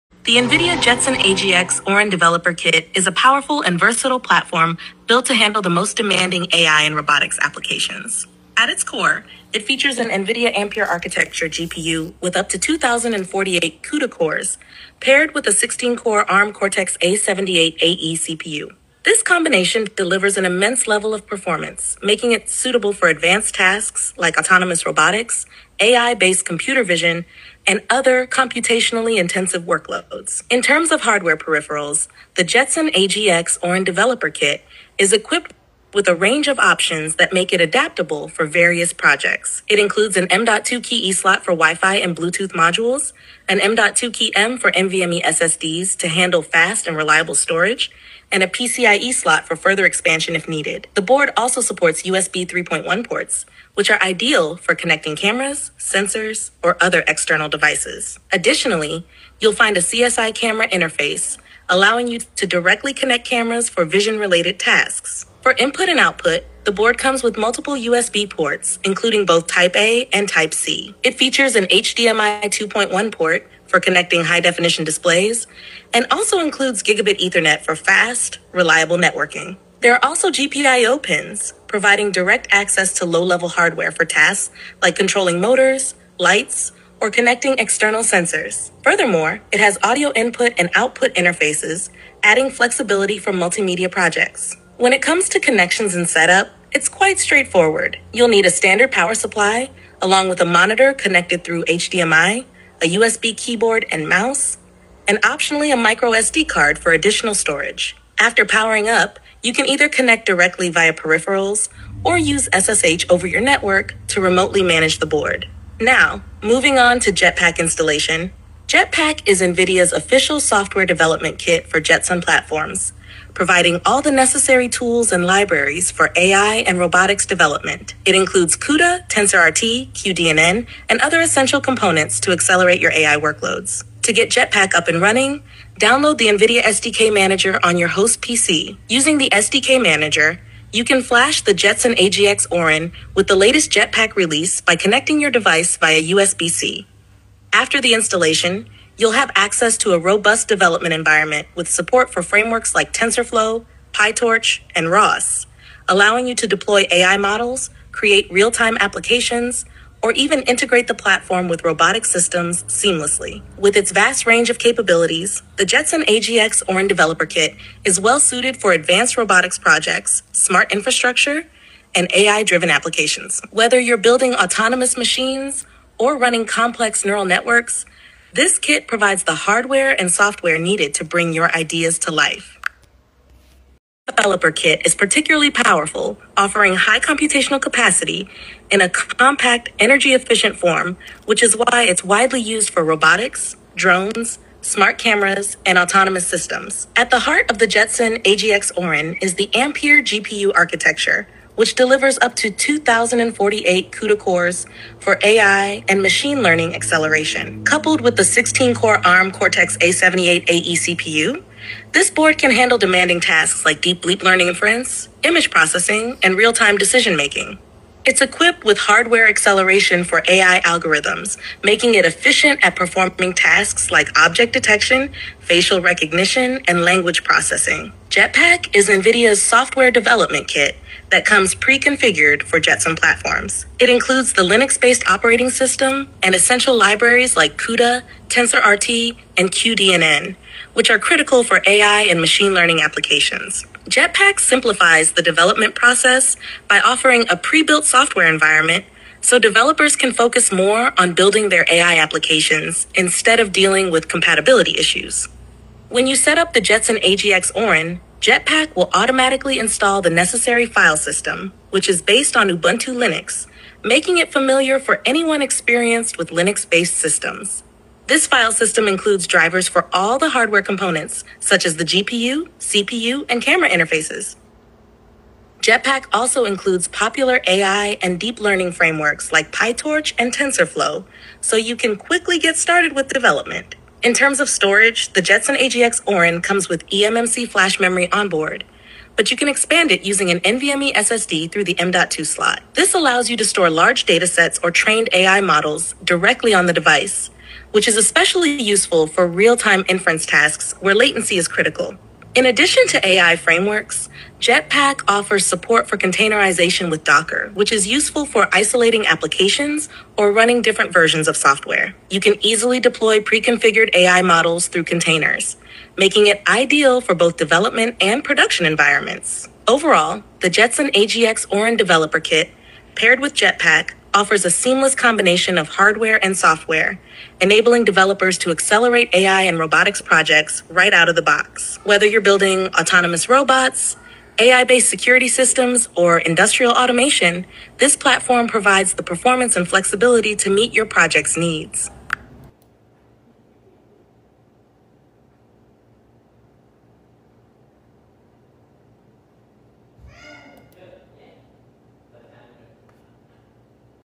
New Nvidia Jetson AGX Orin Getting Started
Please check my confluence page for detailed installation instructions on how to setup Nvidia Jetson AGX Orin for CUDA, Machine Learning, AI, Deep Learning etc.